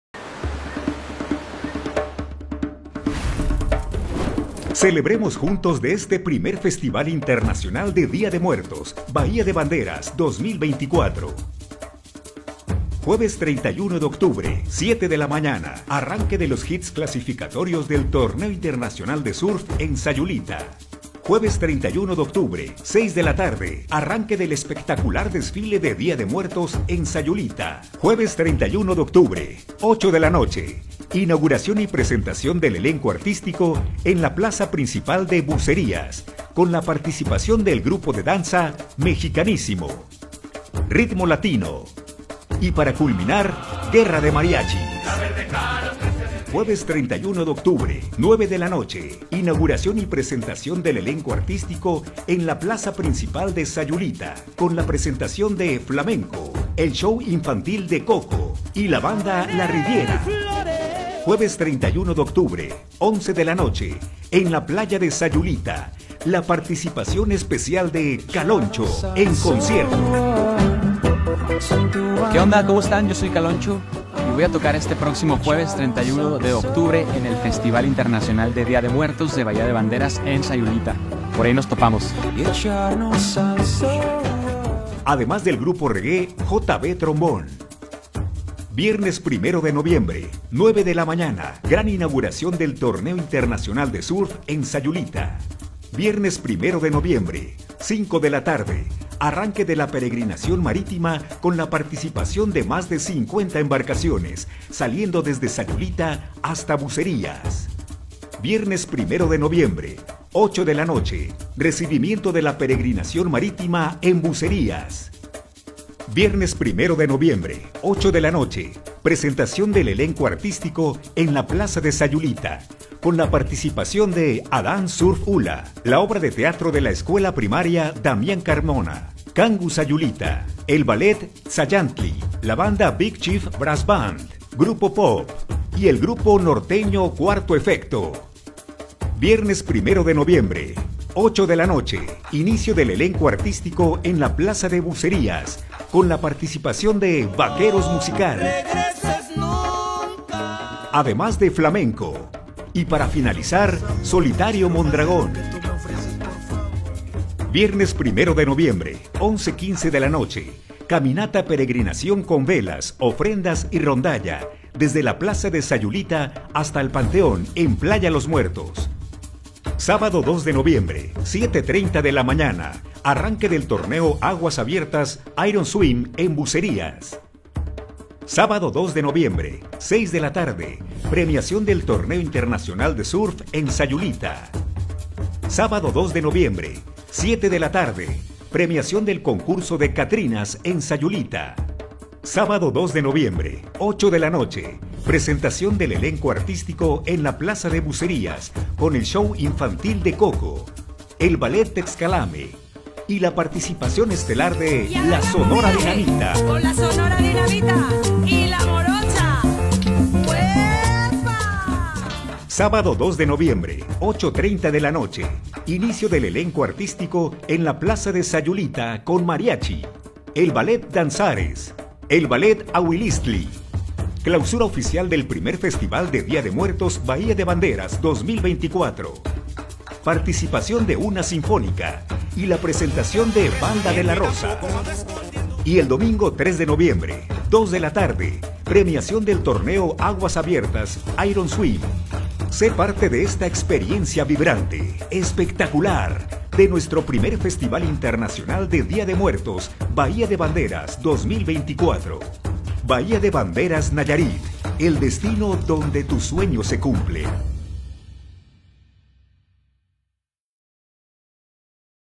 Festival Internacional de Muertos en Bahía de Banderas
A donde ir el dia de muertos? Ven a disfrutar de 2 sedes sayulita y bucerias 1,2 y 3 de noviembre 2024